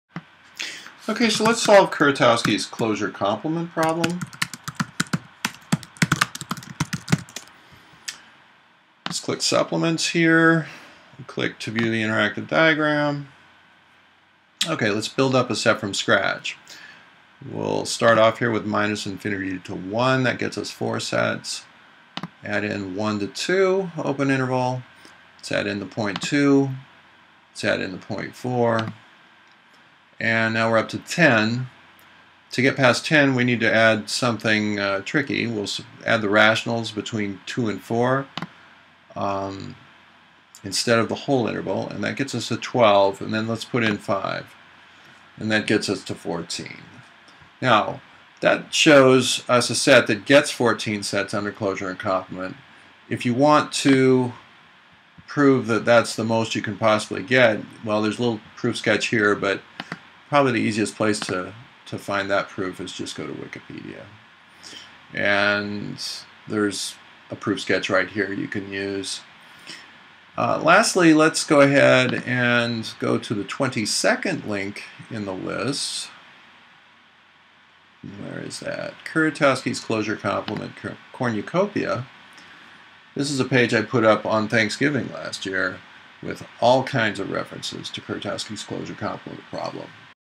Kuratowski's Closure-Complement Problem
In 1920 celebrated Polish mathematician Kazimierz Kuratowski discovered that at most 14 distinct sets can be generated from one starting set in a topological space by applying closure and complement repeatedly.  Given its intrinsic appeal and medium-level difficulty, this problem is now posed to math students all over the world, usually in topology class.  This short video shows a quick and easy way to solve it with the aid of modern websites.